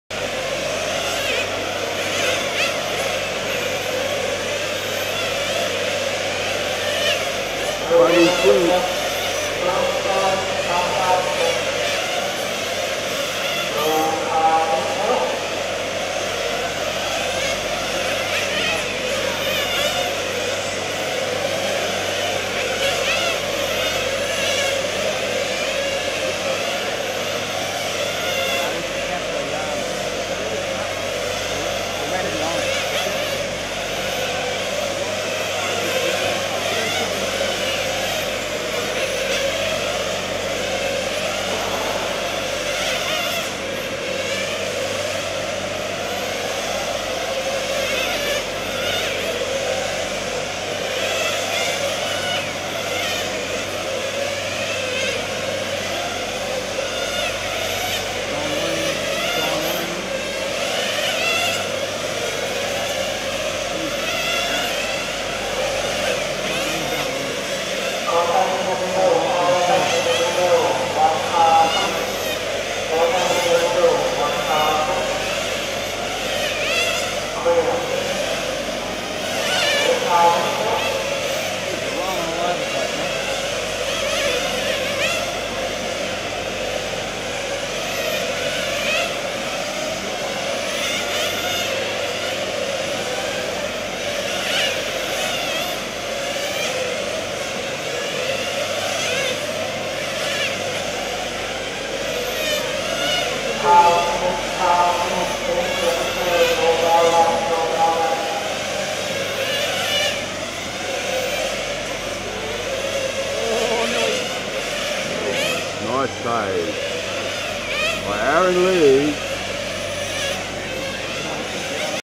Aza with Dario in pursuit
First segment of Aaron in Free practice, red car is Dario Ballestri.